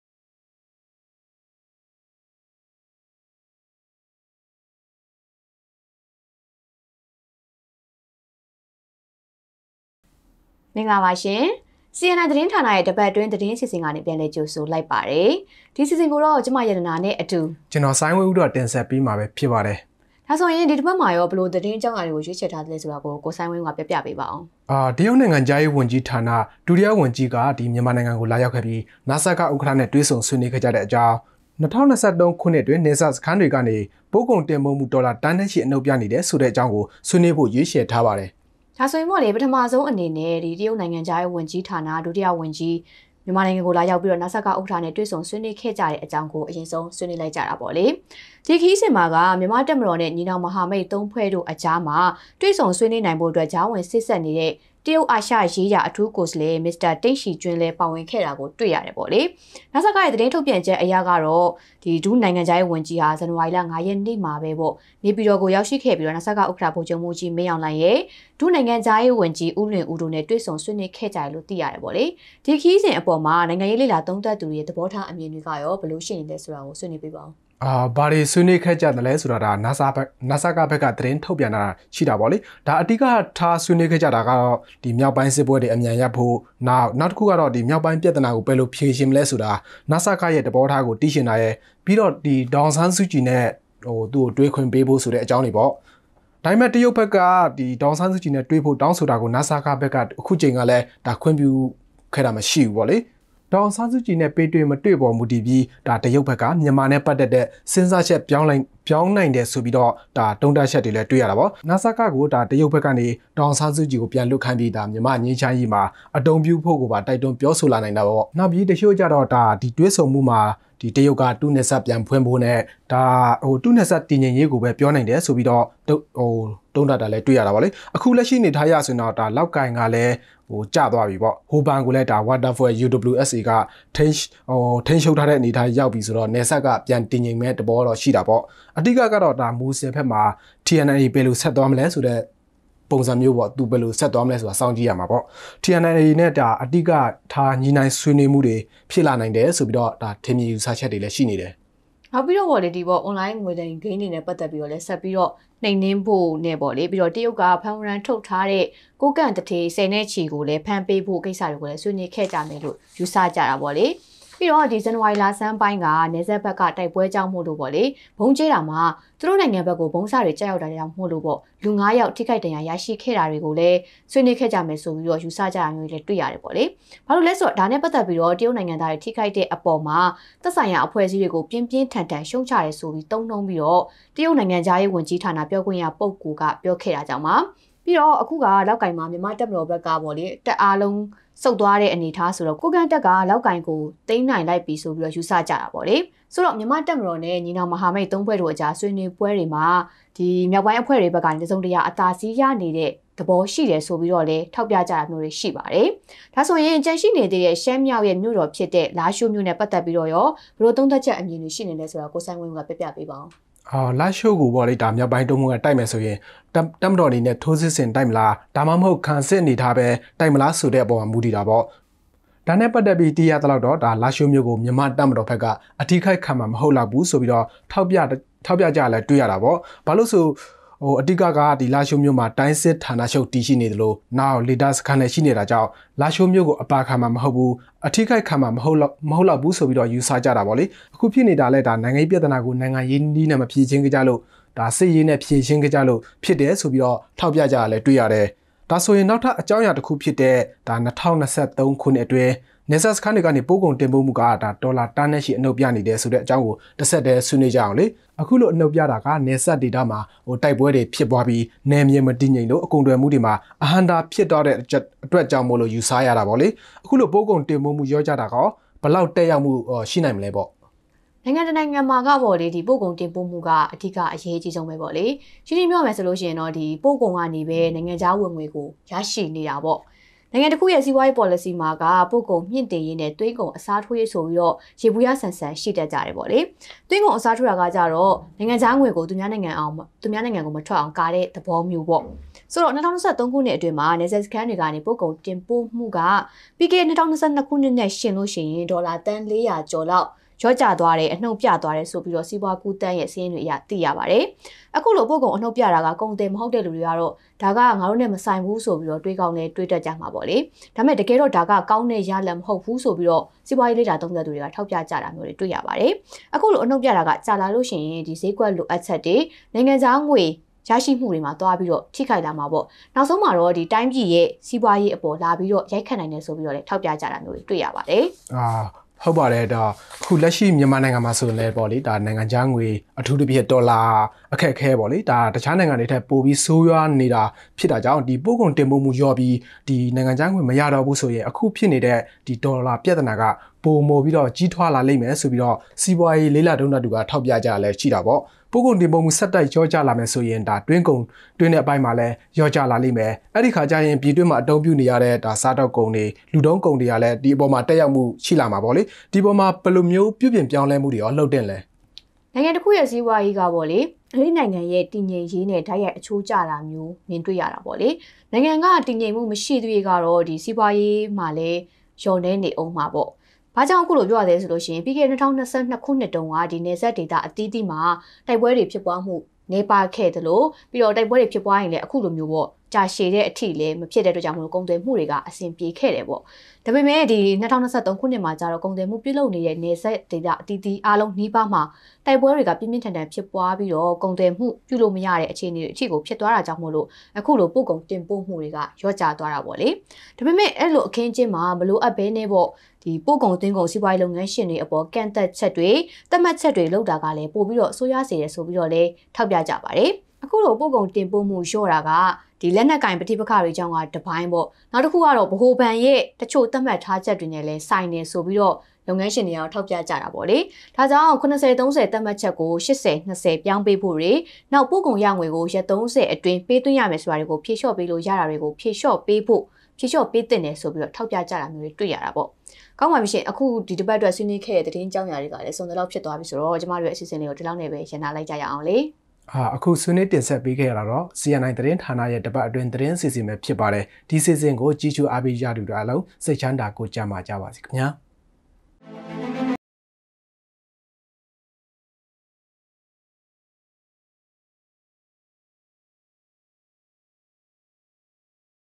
CNI ရဲ့ တစ်ပတ်အတွင်း သတင်းအစီအစဉ်
CNI Weekly News

၂၀၂၄ ခုနှစ်၊ ဇန်နဝါရီ ၁၄

CNI သတင်းဌာနရဲ့ အပတ်စဉ် တနင်္ဂနွေနေ့တိုင်း တင်ဆက်ပေးနေတဲ့ တစ်ပတ်အတွင်း သတင်းအစီအစဉ်ကနေ ကြိုဆိုလိုက်ပါတယ်။

  ဒီတစ်ပတ်မှာတော့ တရုတ်နိုင်ငံခြားရေးဝန်ကြီးဌာန ဒုတိယဝန်ကြီး မြန်မာနိုင်ငံကိုလာရောက်ခဲ့ပြီး နစကဥက္ကဋ္ဌ နဲ့တွေ့ဆုံ ဆွေးနွေးခဲ့ကြတဲ့ အကြောင်းနဲ့  ၂၀၂၃ ခုနှစ်အတွင်း နယ်စပ်စခန်းတွေကနေ ပို့ကုန်တင်ပို့မှု ဒေါ်လာသန်းနဲ့ချီ အနုတ်ပြနေတဲ့ အကြောင်းတွေကို ဆွေးနွေးသွားမှာ ဖြစ်ပါတယ်။